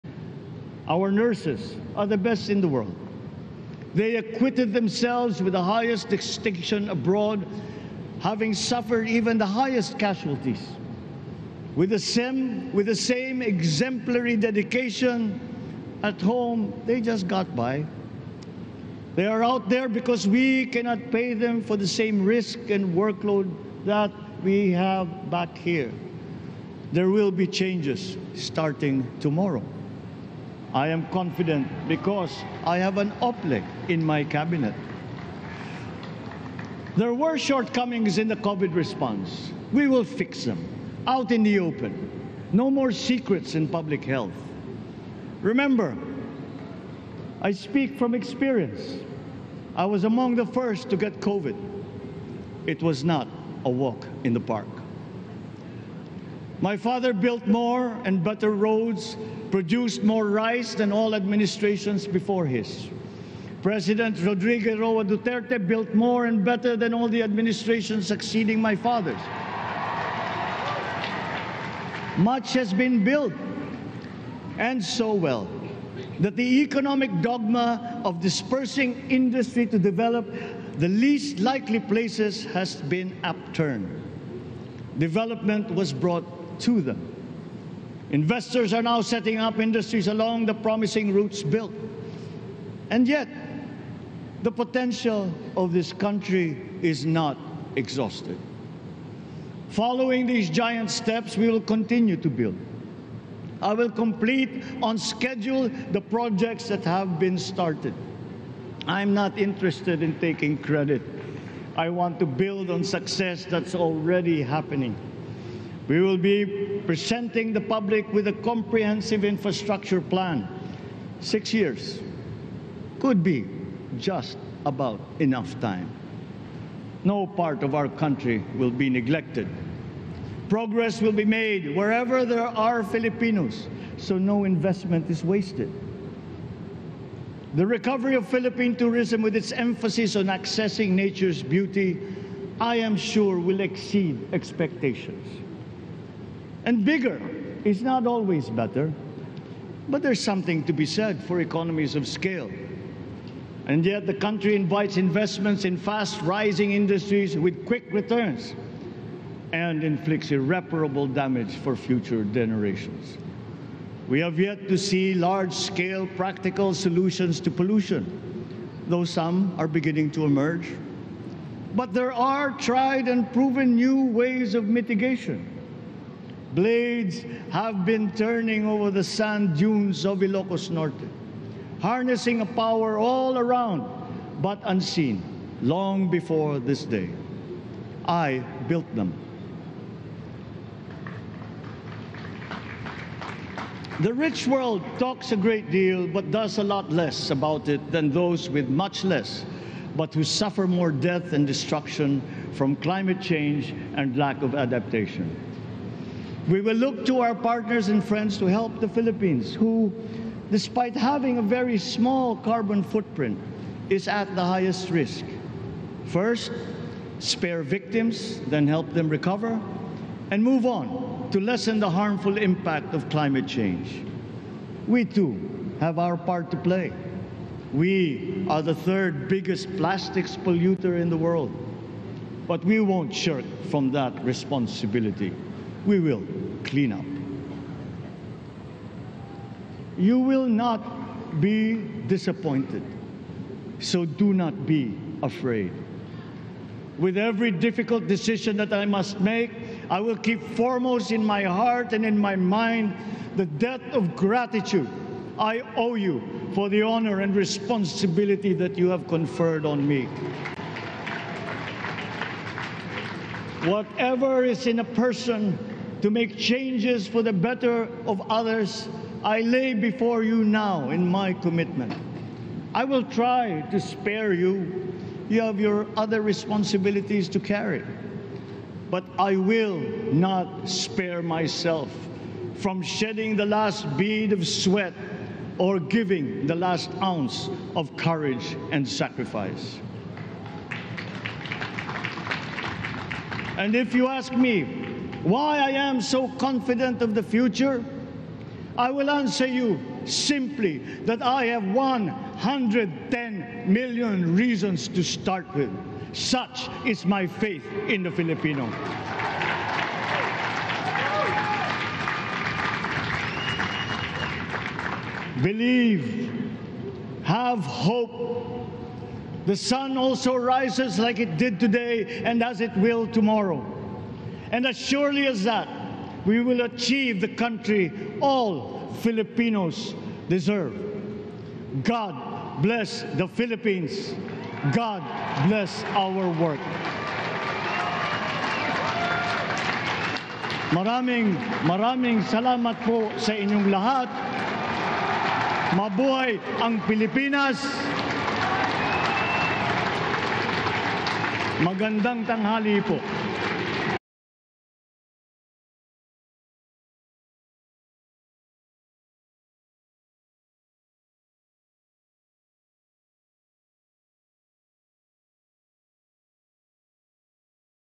Marcos praises Duterte's Build, Build, Build in Inaugural Speech | ANC
The Marcos Inaugural: Ferdinand 'Bongbong' Marcos Jr. takes his oath as the 17th President of the Philippines at the National Museum.

This is ANC's Special Coverage of the Marcos Inauguration.